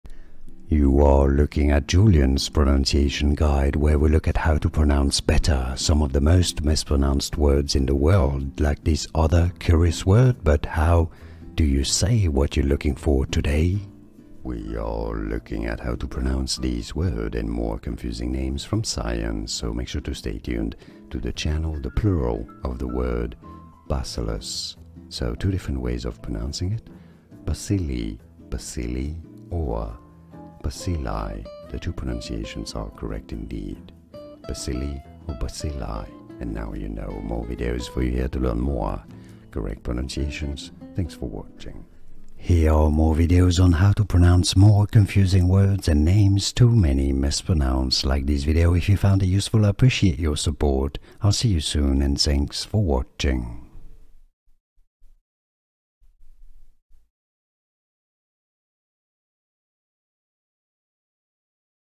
How to Pronounce Bacilli (correctly!)
Welcome! Here you will hear How to Pronounce Bacilli (correctly!) i.e. the “correct” pronunciation.

You can skip the intro through the time stamps below:
00:00 - Pronunciation Intro
00:15 - How to Pronounce Bacilli (correctly!)